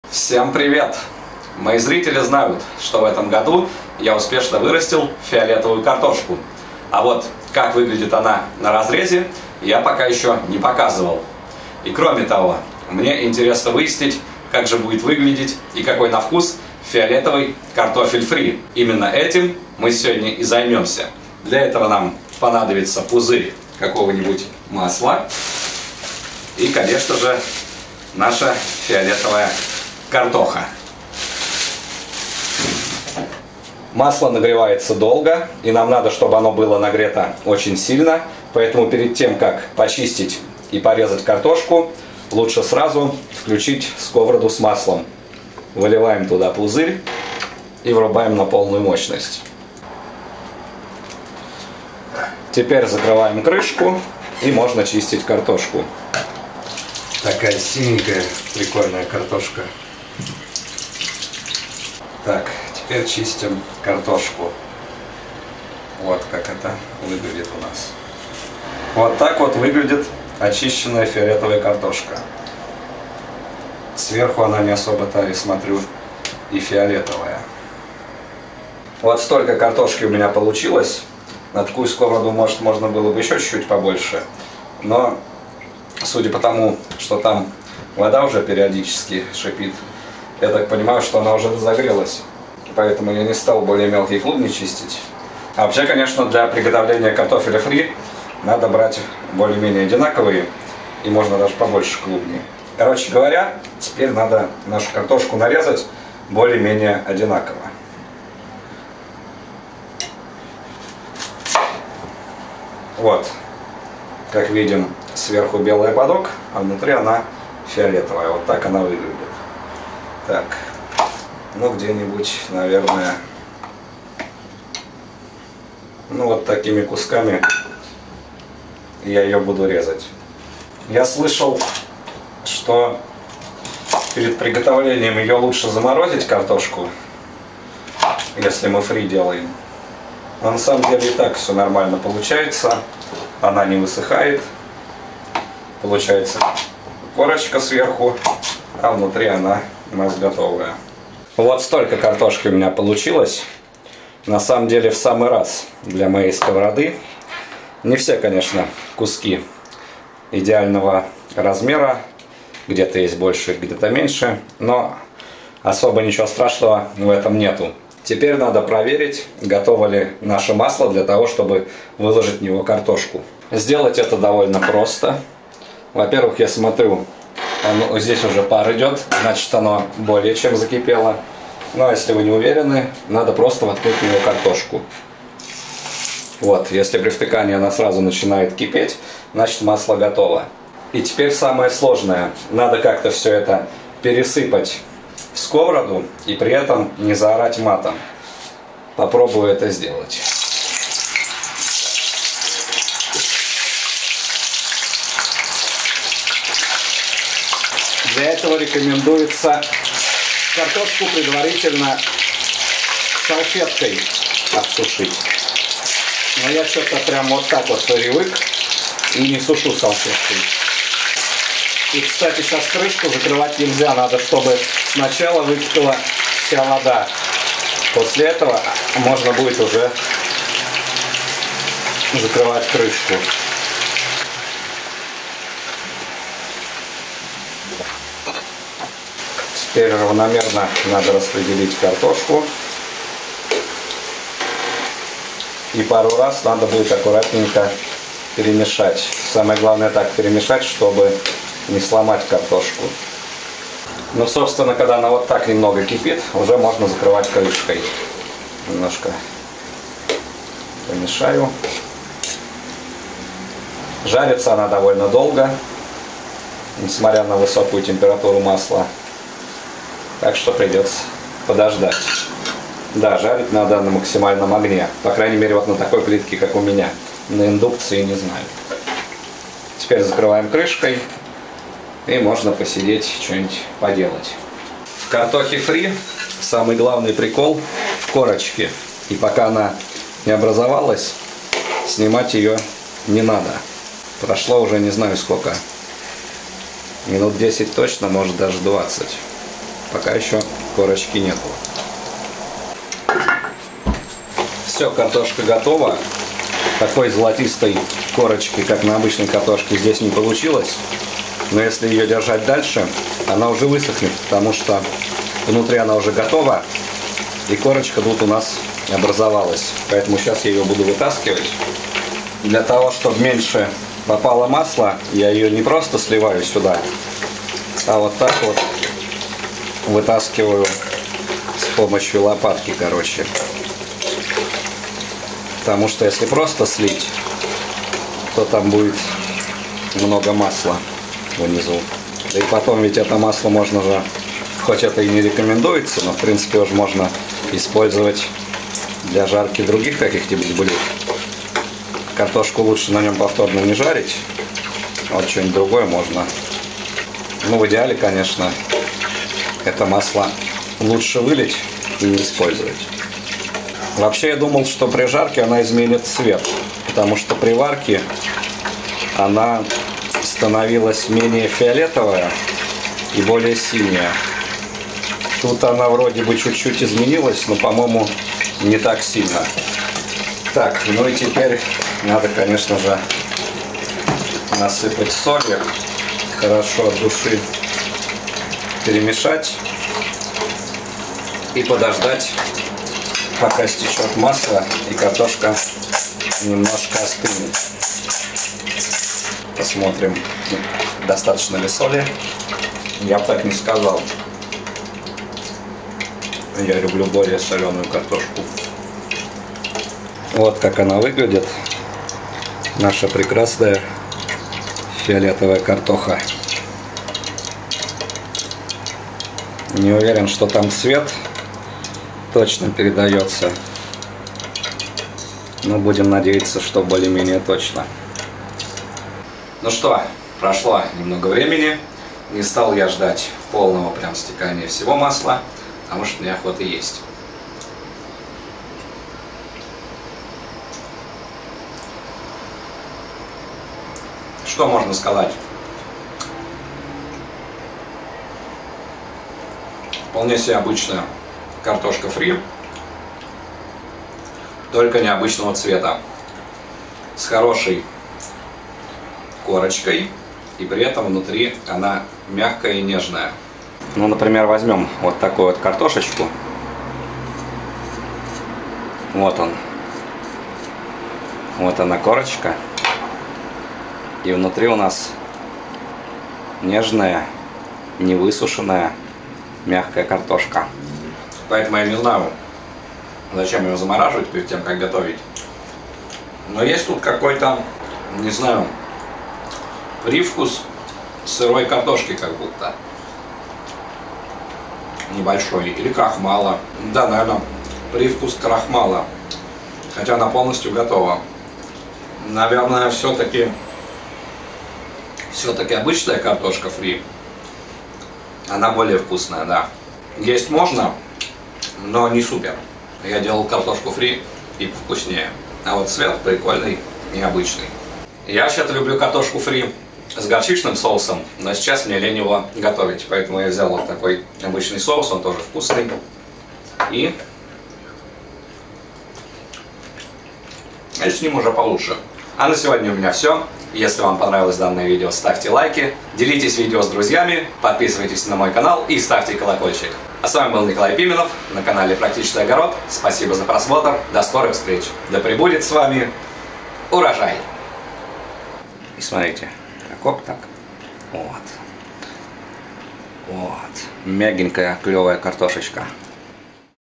Фиолетовый картофель фри в домашних условиях
Сегодня я буду готовить фиолетовый картофель фри в домашних условиях и попробую его на вкус!